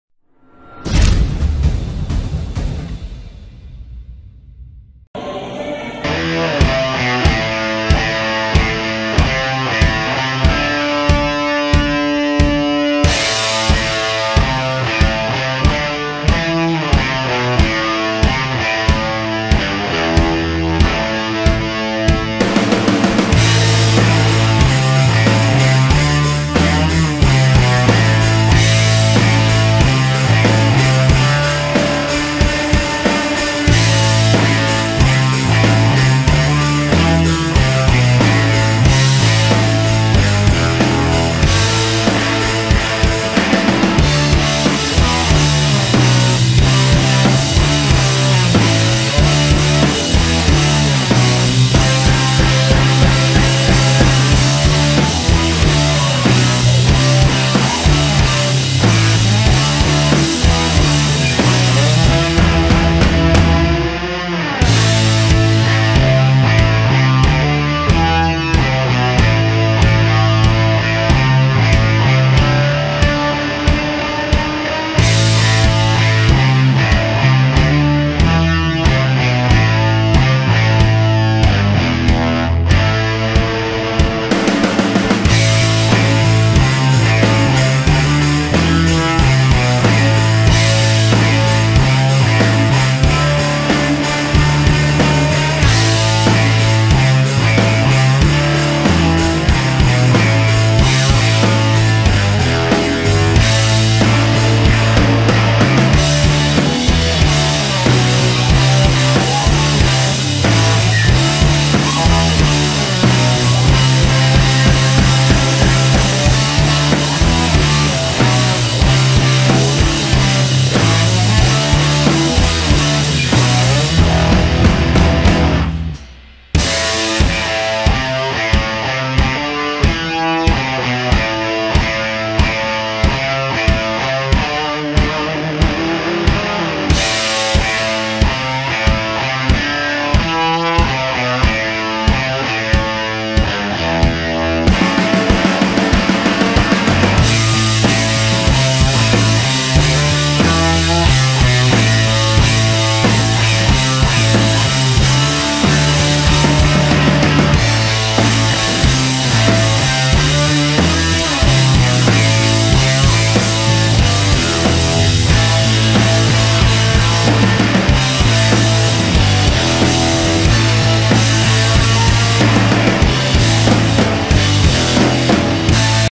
Hadi Choopan - Couldn't walk after Insane legs workout - Mr Olympia 2020
Hadi Choopan - Couldn't walk after legs training - Mr Olympia 2020

Your subscription is our huge motivation for us !!